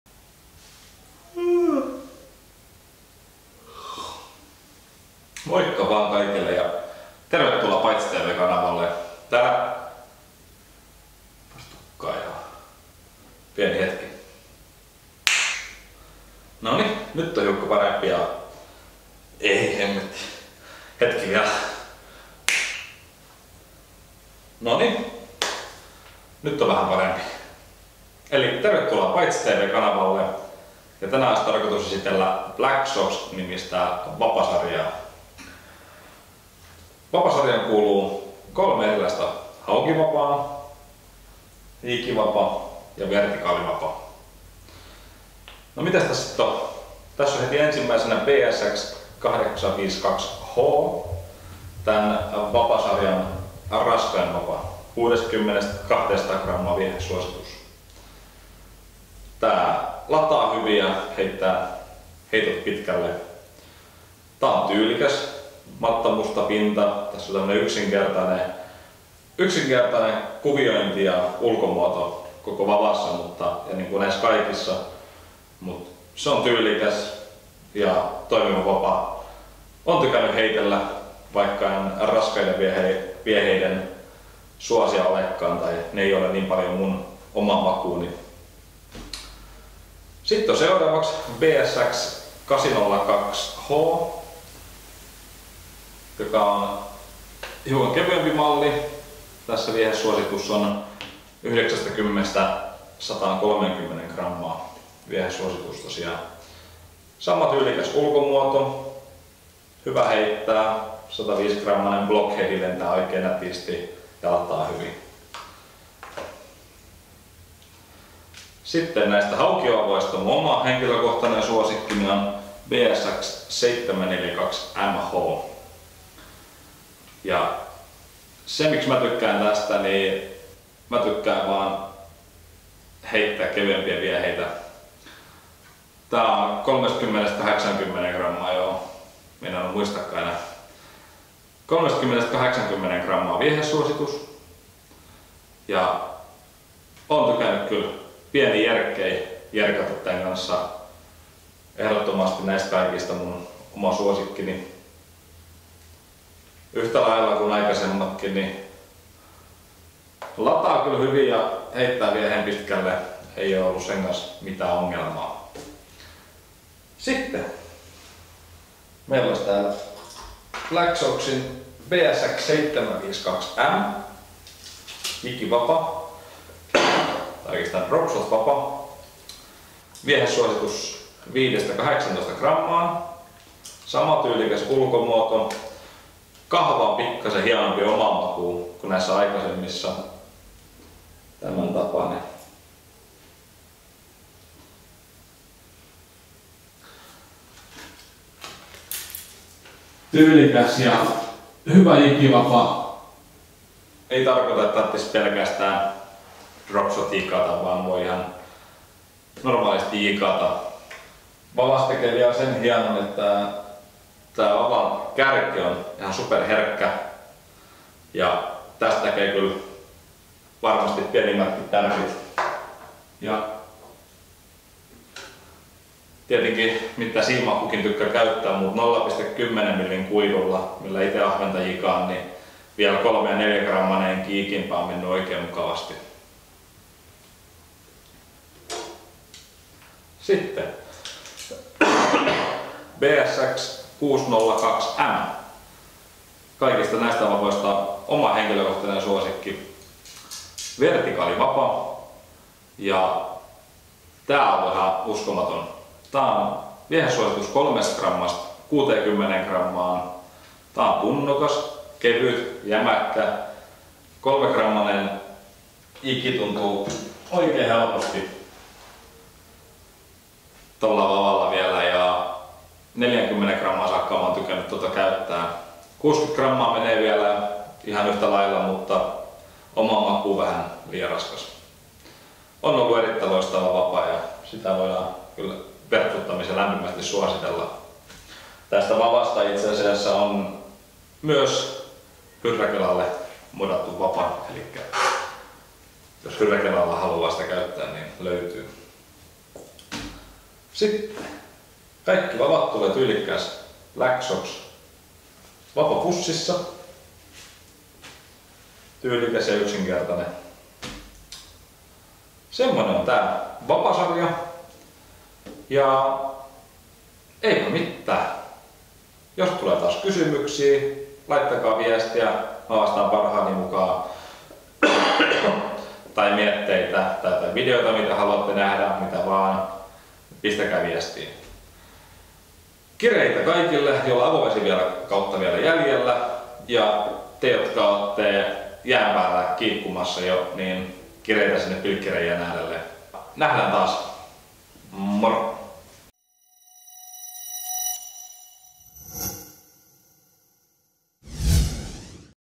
Blacksox vapasarjan esittely
Blacksox-vapasarja on tehty yhteistyössä Korealaisen Geunsun N.S ja Scandinavian Tackle:n kanssa. Vapasarjan ideana oli tehdä yksinkertainen, toimiva ja siisti vapasarja jossa on vaihtoehtoja tämän päivän kalastusmuotoihin pohjoismaissa. Suunnittelussa on vahvasti ollut mukana Team Scandinavian Tackle joka on speksannut vapojen toiminnan, viehepainon ja muut yksityiskohdat. Vapasarjan yksityiskohdat hivelevät silmää ja ne on pyritty pitämään yksinkertaisena. Vavat toimitetaan hienossa kuljetuspussisa. Takuu 3vuotta.

Videolla esitetyt vavat:

Blacksox BSX 852H
Blacksox BSX 802H
Blacksox BSX 742MH
Blacksox BSX 602M SPIN
Blacksox BSX 752M

Blacksox-vapasarjan vapoja voi ostaa sivulta.